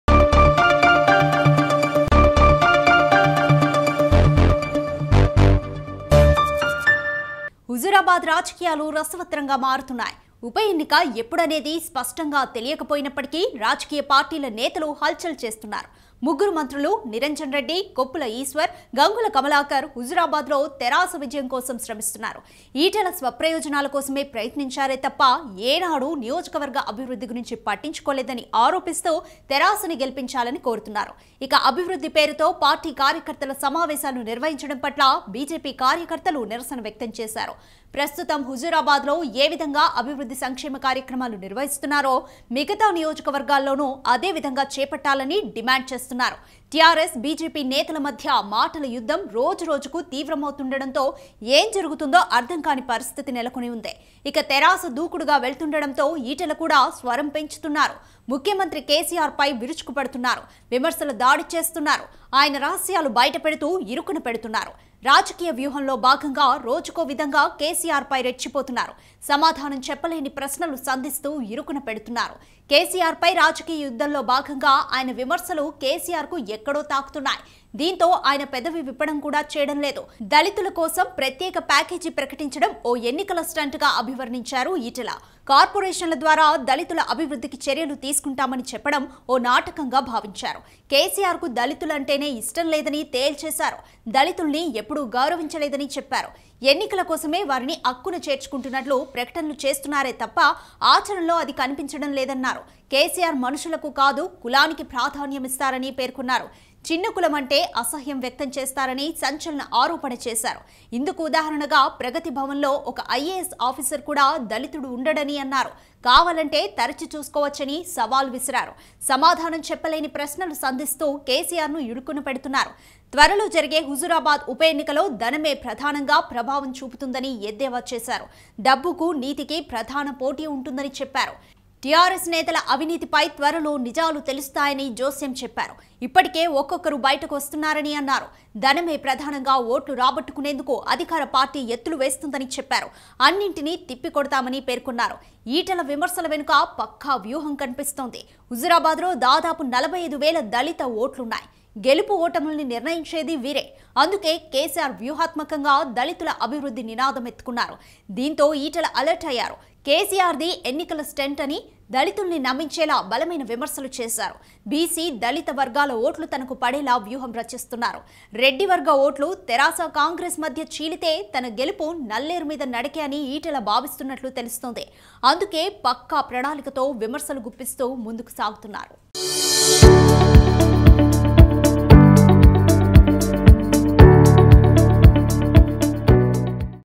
BJP Etela Rajender New Strategy | Huzurabad Elections | CM KCR Vs Etela Rajender | ALO TV
BJP Etela Rajender New Strategy | Huzurabad Elections | CM KCR Vs Etela Rajender